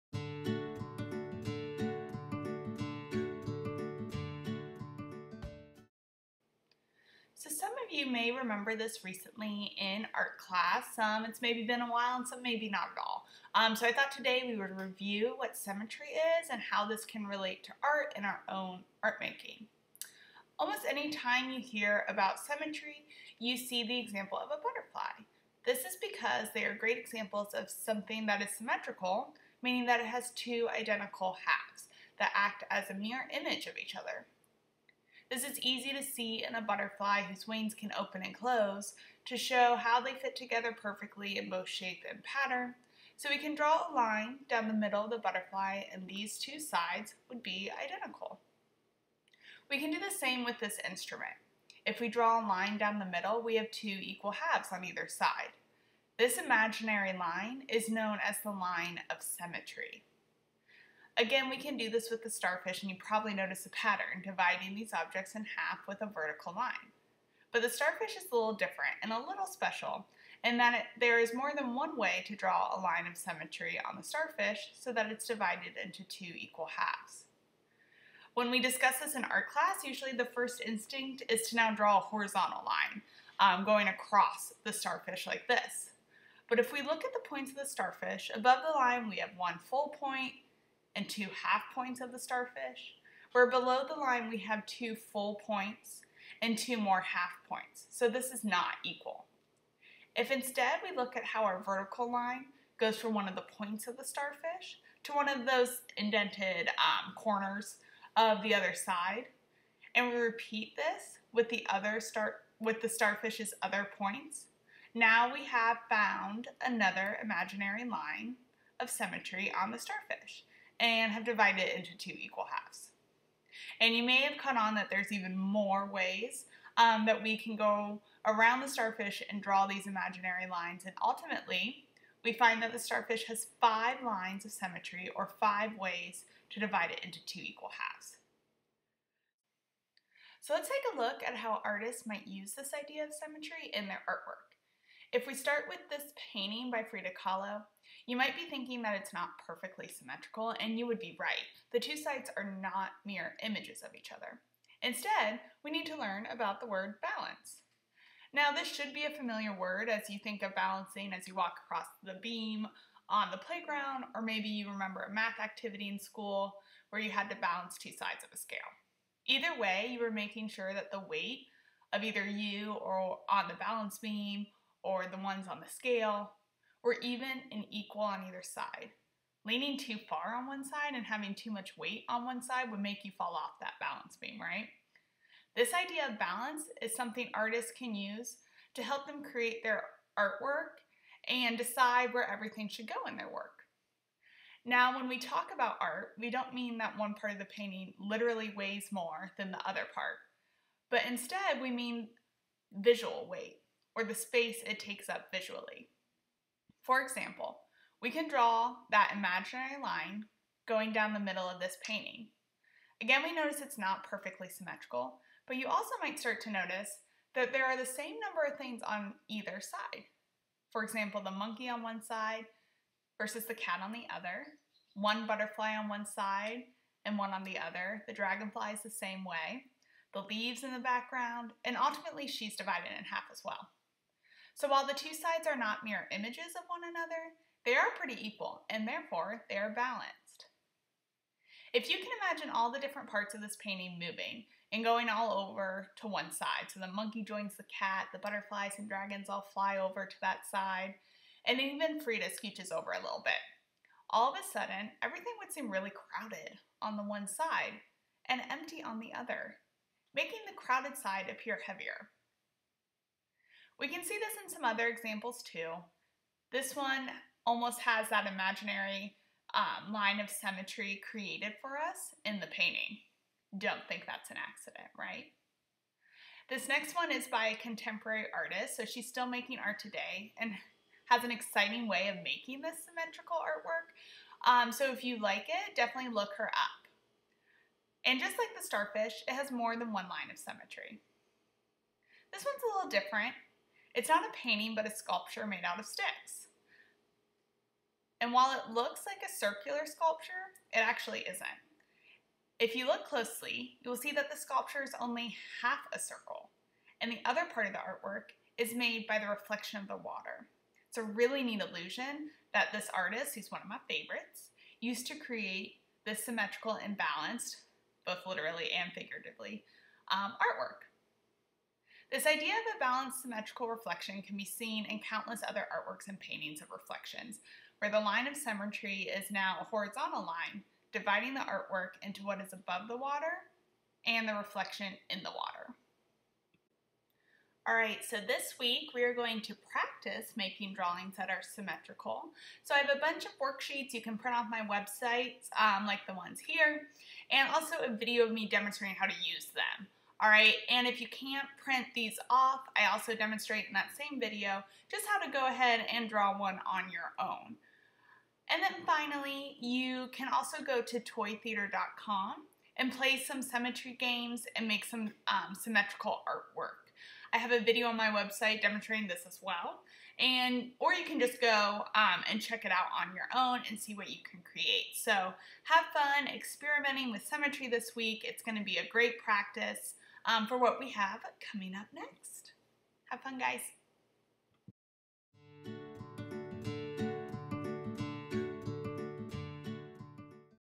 Symmetry Review
Watch this video for a review of symmetry and how artists can use symmetry in their artwork to create balance.